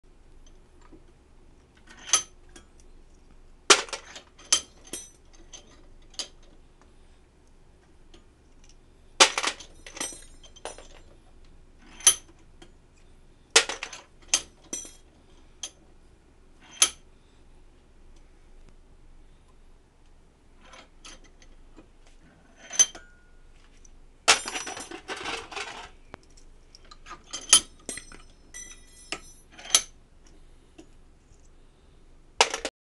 Removing ammo from links/belts
So I was looking up a way to De-link ammo that was easy and fast. Well all I did was waste my time I tried a few different methods till I figured this out so just thought I might save someone else some time .